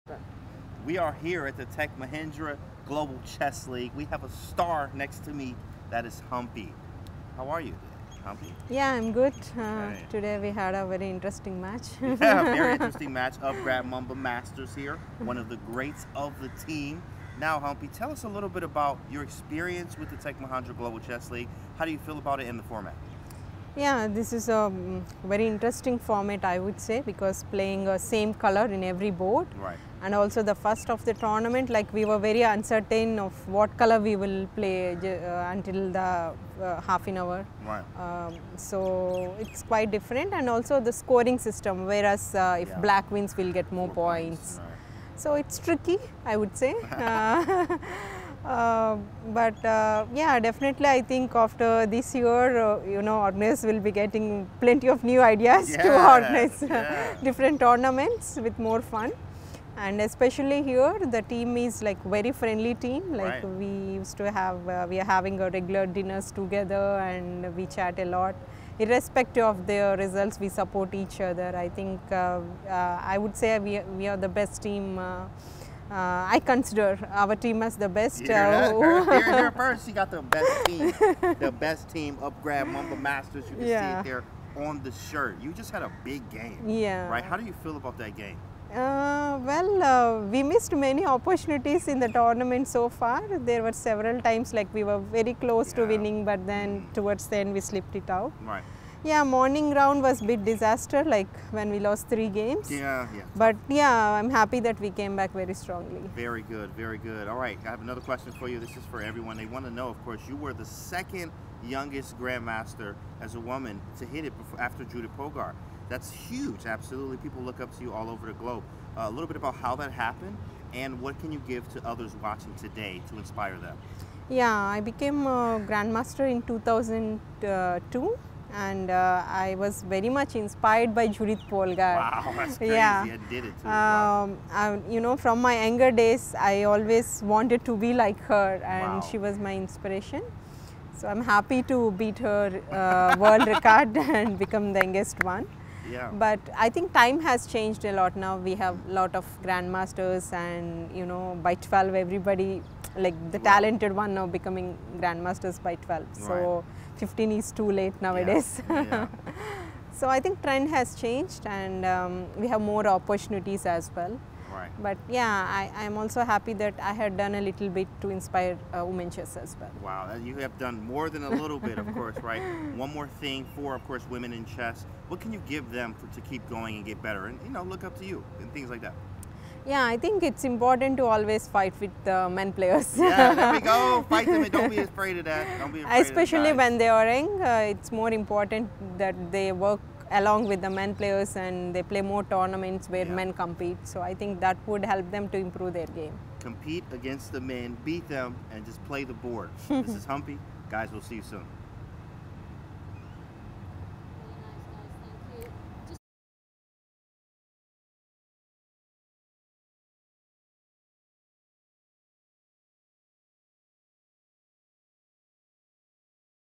Koneru Humpy talks about GCL🔥
James Canty III sits down with Koneru Humpy for a candid chat to talk about her experience at the Tech Mahindra Global Chess League so far💯